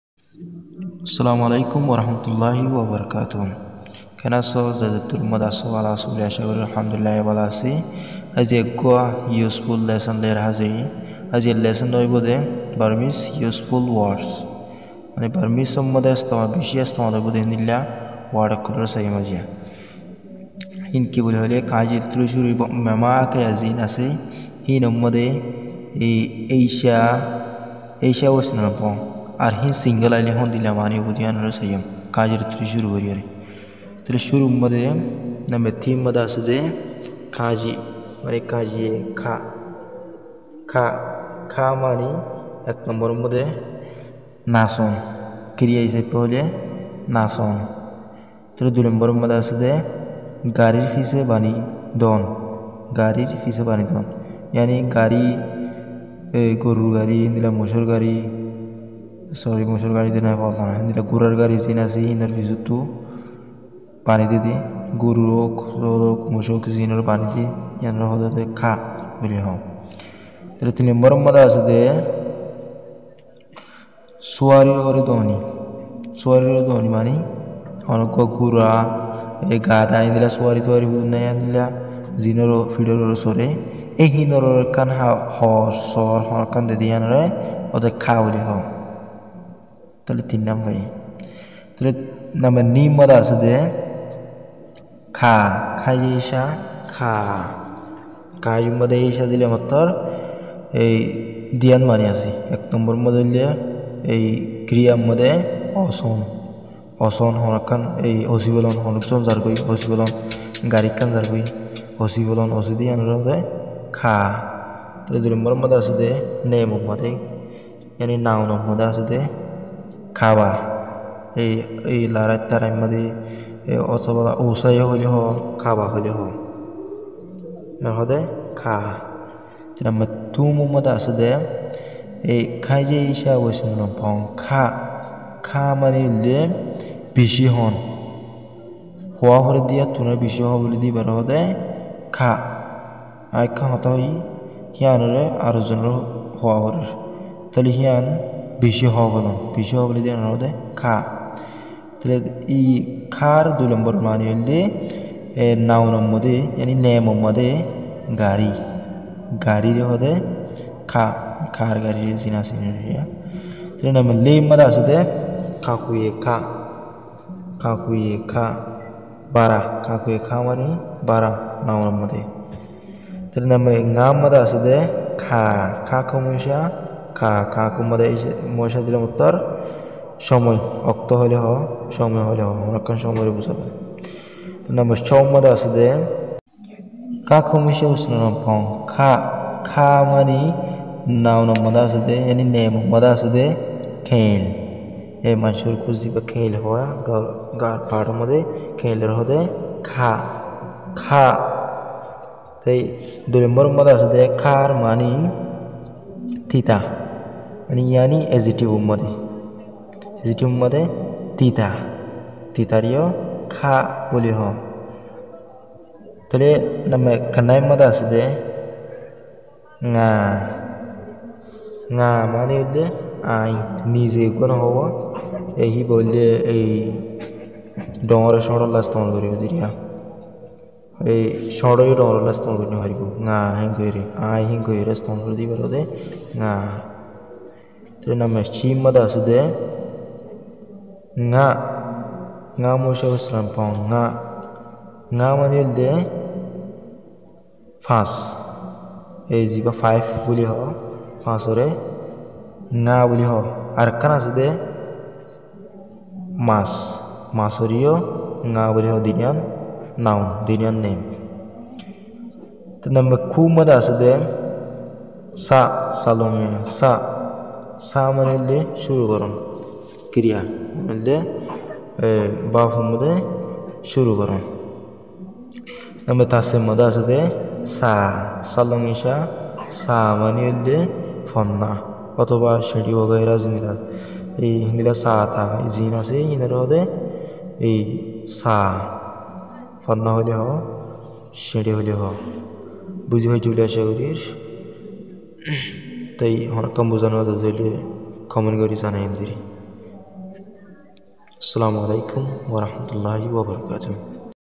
Burmese useful words#part-1
Burmese useful words. The words will be very useful for those who are enthusiastic to learn Burmese.